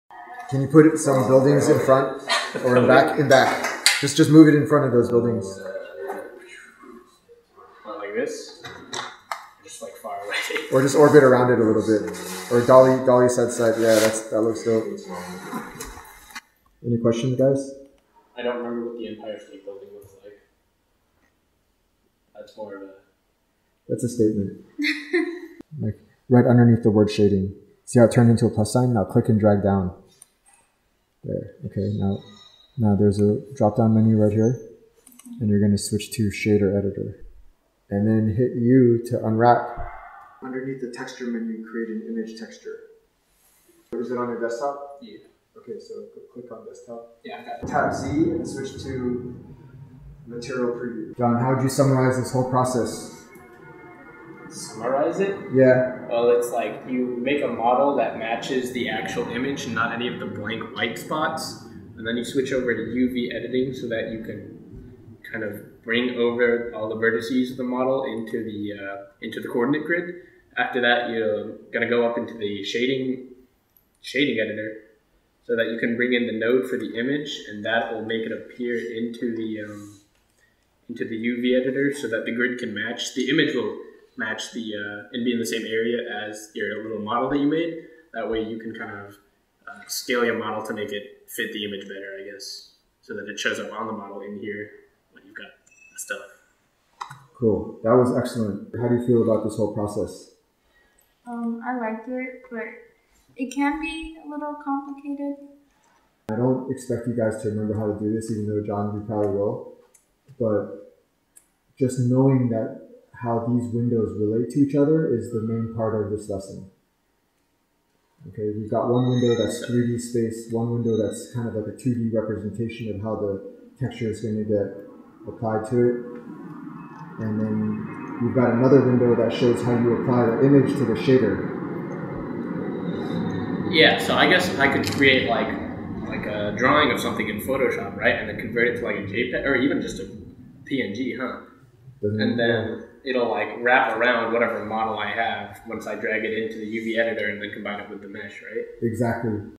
Blender Projection Mapping Tutorial with Interns
Shar and John worked together to create this awesome 3D robot!